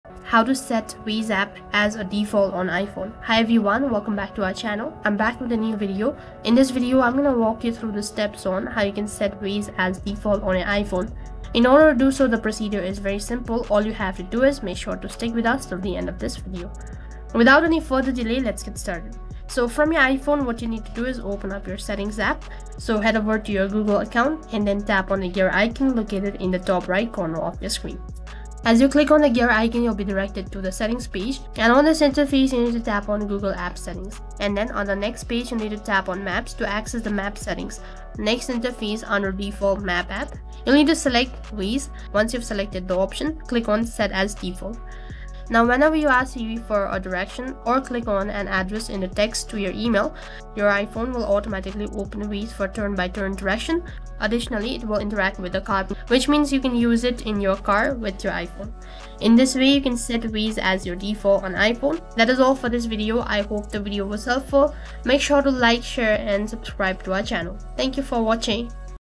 How to Set Waze as your Default Navigation app on iPhone !! Make Waze my Default Navigation App 2023
In this video I'll walk you through the steps on how you can set waze as your default navigation app on iphone.
Chapters / Timestamps
0:00 Intro
0:20  set as default.
1:02 Outro.